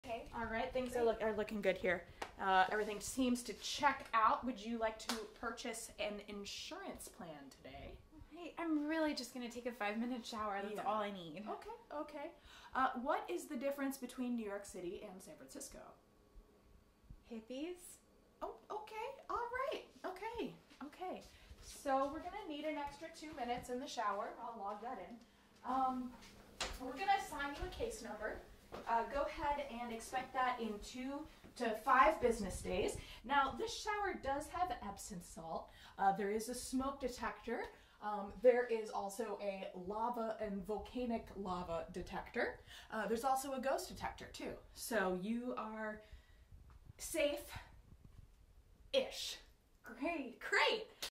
Story of my Life by Edna: MAYBE SHOWER? Episode 13
Finishing the questionnaire for my shower experience.
Hippies?

Featuring Guinevere Q.
Filmed and edited by Jason Young
February 2020